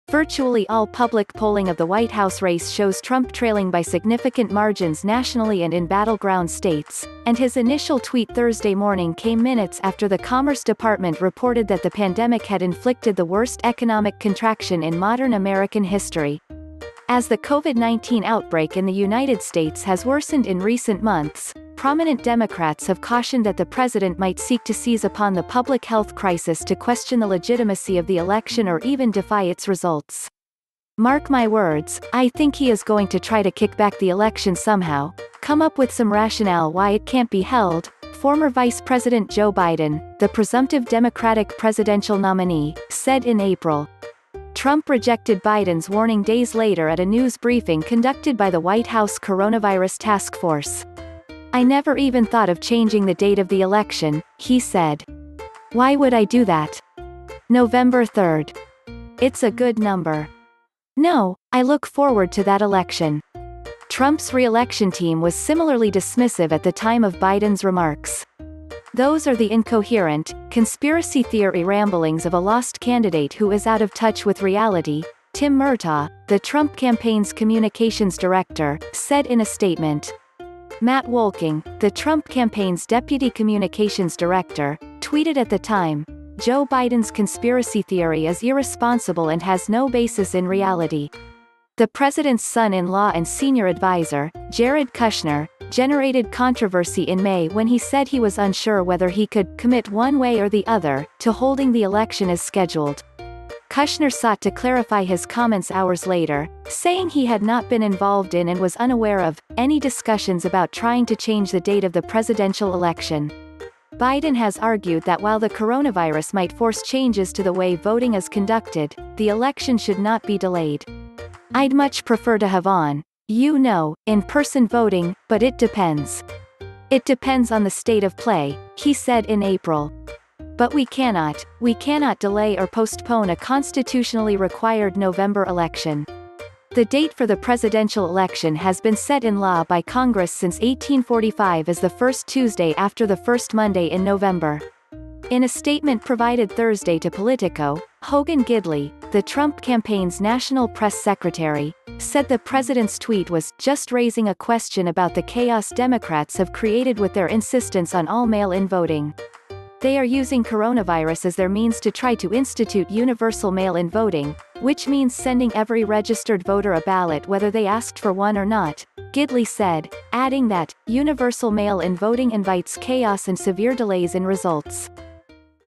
Trump refuses to back down on suggestion of election delay
Virtually all public polling of the White House race shows Trump trailing by significant margins nationally and in battleground states, and his initial tweet Thursday morning came minutes after the Commerce Department reported that the pandemic had inflicted the worst economic contraction in modern American history. As the Covid-19 outbreak in the United States has worsened in recent months, prominent Democrats have cautioned that the president might seek to seize upon the public health crisis to question the legitimacy of the election or even defy its results.“Mark my words, I think he is going to try to kick back the election somehow, come up with some rationale why it can’t be held,” former Vice President Joe Biden, the presumptive Democratic presidential nominee, said in April. Trump rejected Biden’s warning days later at a news briefing conducted by the White House coronavirus task force.“I never even thought of changing the date of the election,” he said. “Why would I do that? November 3rd. It’s a good number. No, I look forward to that election.”Trump’s reelection team was similarly dismissive at the time of Biden’s remarks.“Those are the incoherent, conspiracy theory ramblings of a lost candidate who is out of touch with reality,” Tim Murtaugh, the Trump campaign’s communications director, said in a statement. Matt Wolking, the Trump campaign’s deputy communications director, tweeted at the time: “Joe Biden’s conspiracy theory is irresponsible and has no basis in reality.”The president’s son-in-law and senior adviser, Jared Kushner, generated controversy in May when he said he was unsure whether he could “commit one way or the other” to holding the election as scheduled. Kushner sought to clarify his comments hours later, saying he had not been involved in and was unaware of “any discussions about trying to change the date of the presidential election.”Biden has argued that while the coronavirus might force changes to the way voting is conducted, the election should not be delayed.“I’d much prefer to have on — you know, in-person voting, but it depends. It depends on the state of play,” he said in April. “But we cannot, we cannot delay or postpone a constitutionally required November election.”The date for the presidential election has been set in law by Congress since 1845 as the first Tuesday after the first Monday in November. In a statement provided Thursday to POLITICO, Hogan Gidley, the Trump campaign’s national press secretary, said the president’s tweet was “just raising a question about the chaos Democrats have created with their insistence on all mail-in voting.”“They are using coronavirus as their means to try to institute universal mail-in voting, which means sending every registered voter a ballot whether they asked for one or not,” Gidley said, adding that “universal mail-in voting invites chaos and severe delays in results.